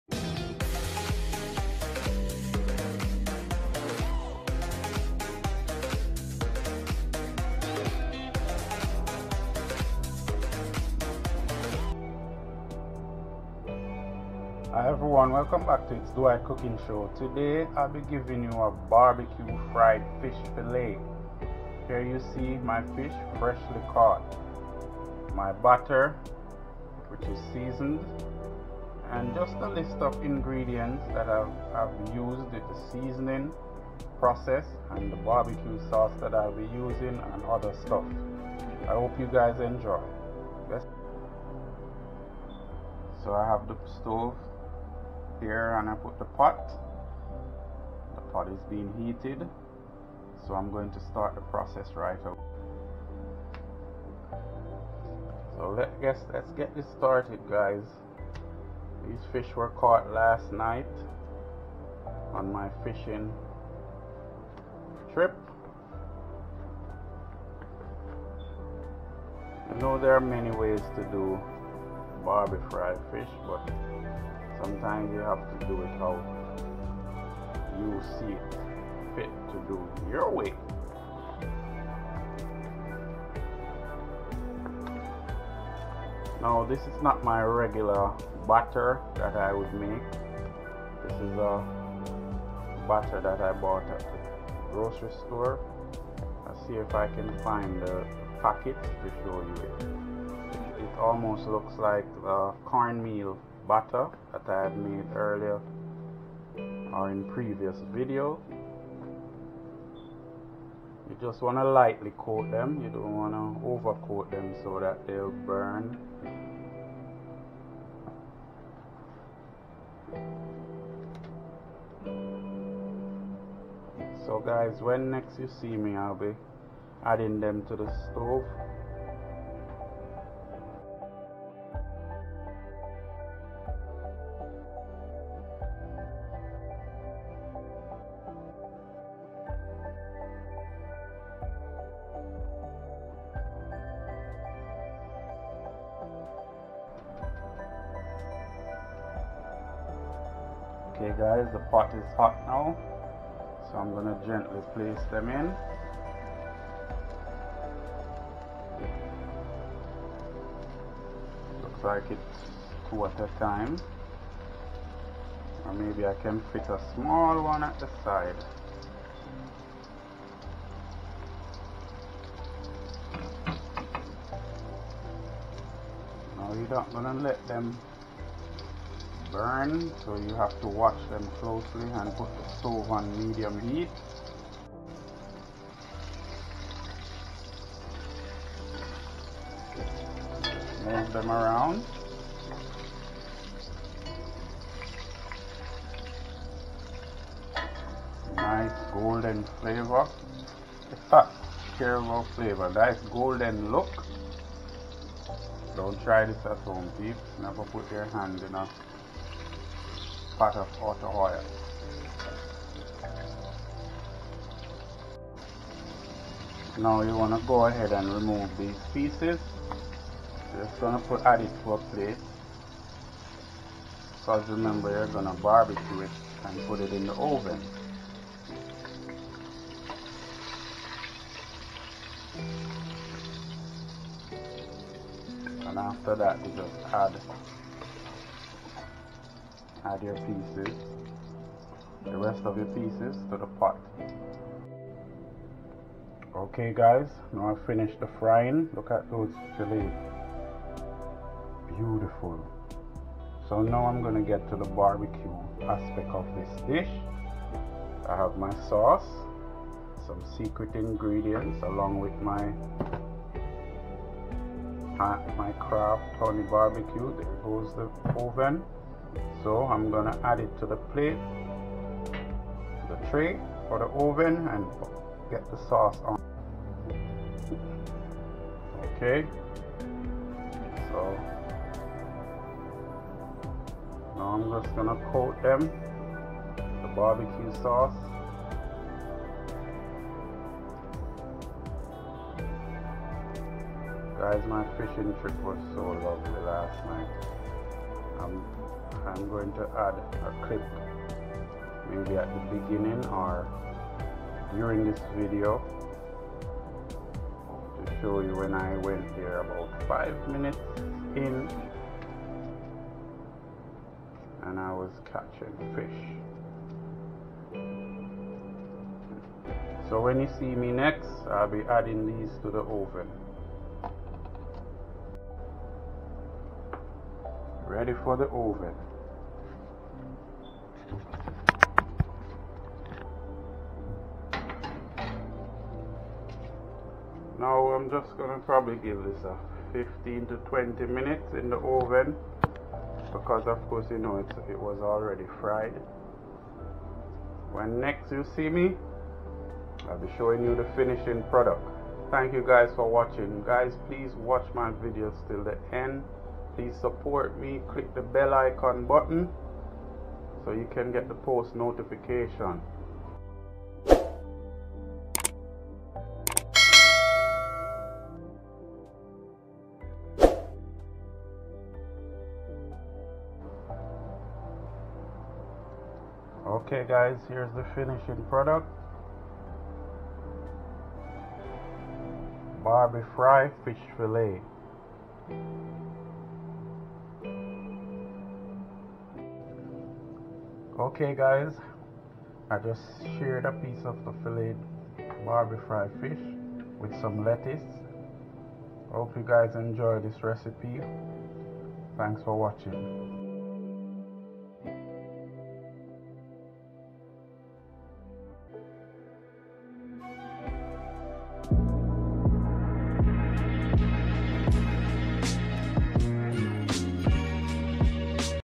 OVEN BAKED FISH | BBQ FRY FISH RECIPE | ITS DWIGHT COOKING SHOW
Hello everyone its Dwight Cooking Show with another amazing recipe BBQ Fried Fish. Oven Baked Fish recipe